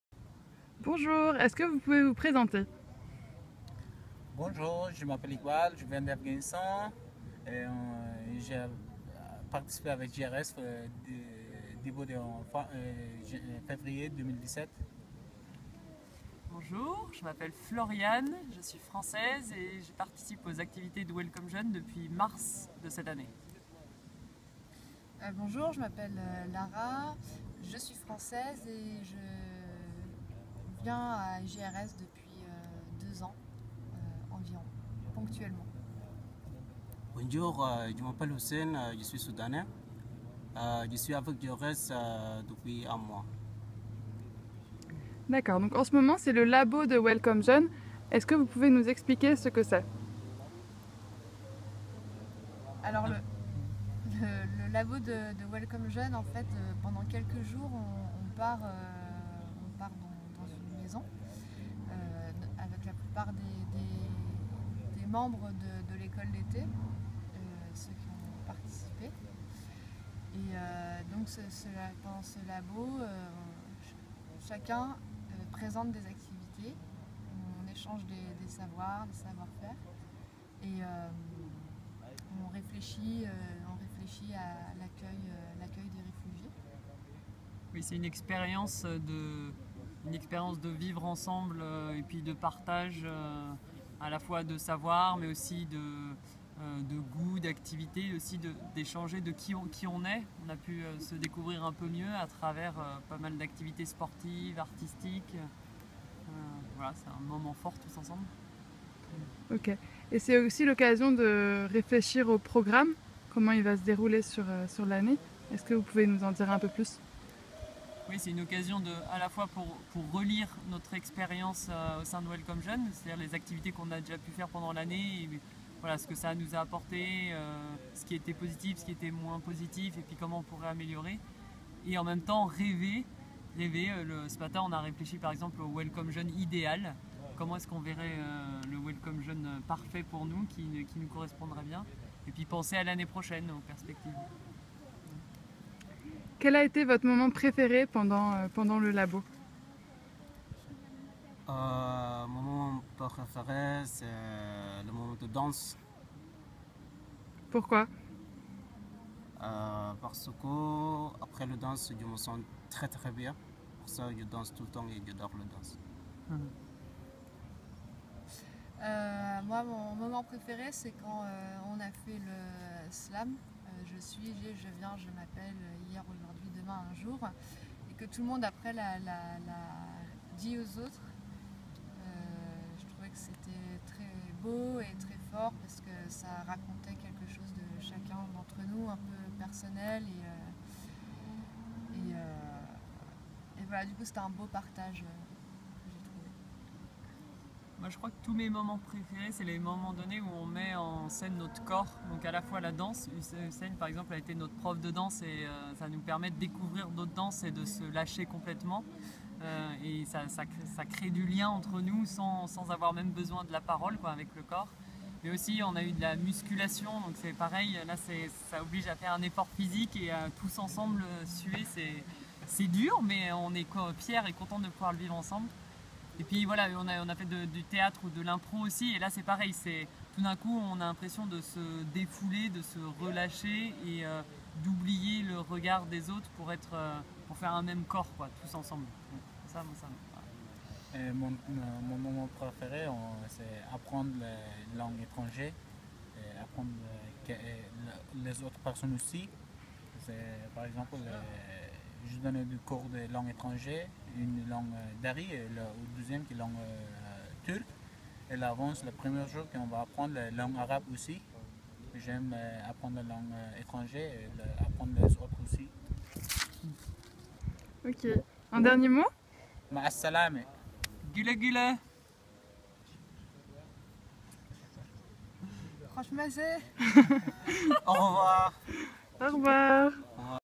Au Labo JRS Jeunes 2017
Iqbal, Floriane, Lara et Hussain content leur expérience au Labo JRS Jeunes !